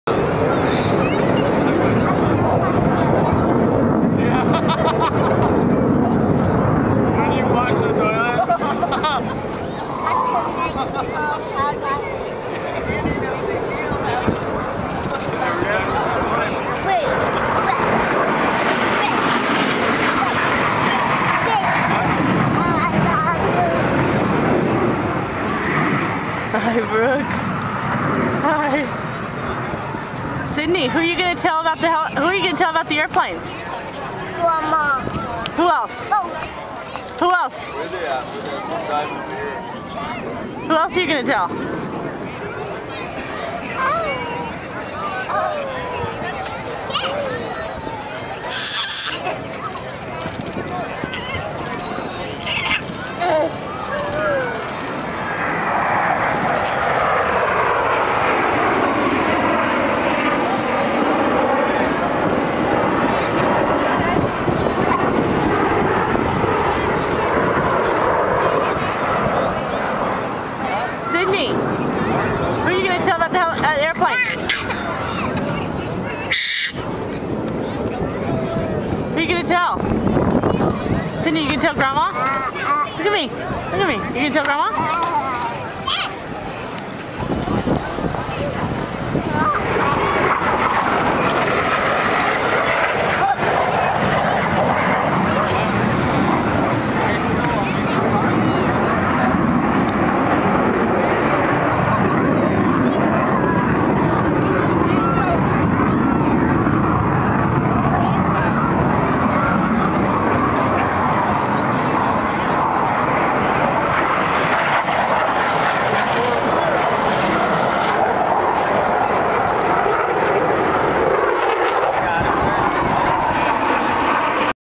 Sydney and Brooke At The Air Show
Sydney, Brooke and their annoying mother who continues to ask Sydney the same question over and over